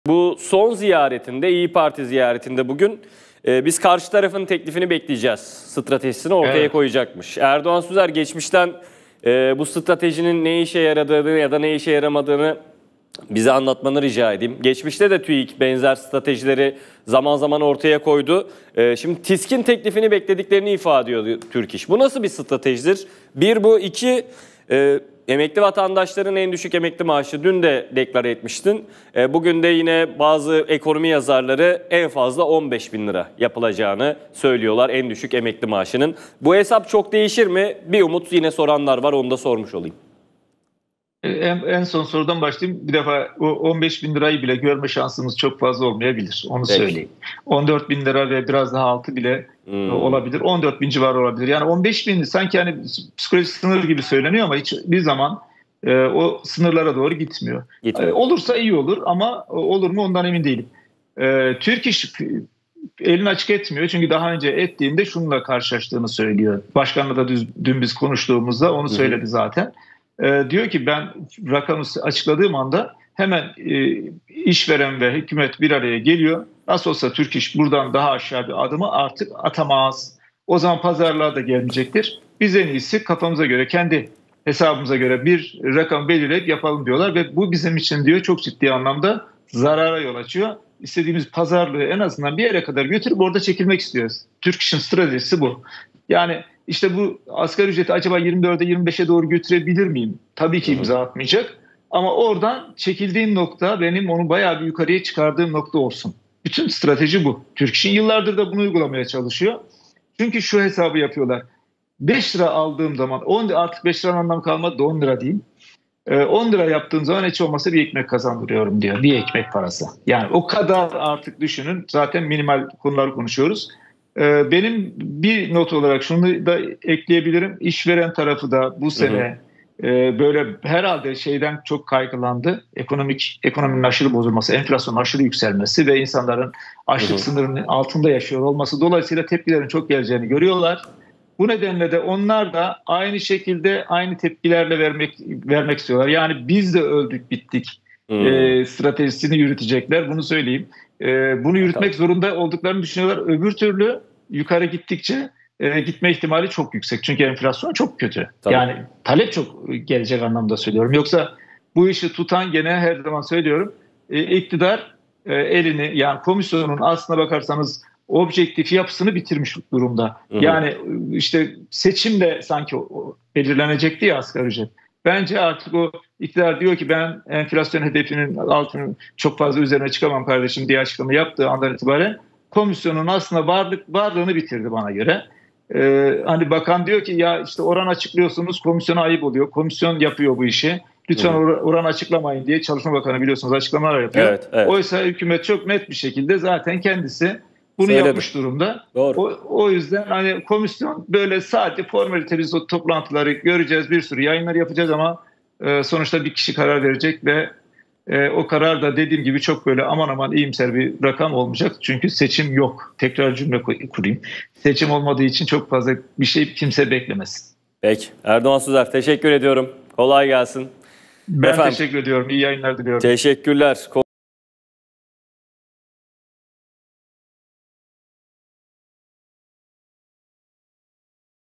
'15 Bin TL Zor...' Erdoğan Süzer En Düşük Emekli Maaşı Tahminini Açıkladı! Zam Ne Kadar Olacak?
Digiturk: 60
D-smart: 93
Turkcell TV Plus: 69
Tivibu: 69
Türksat Kablo TV: 76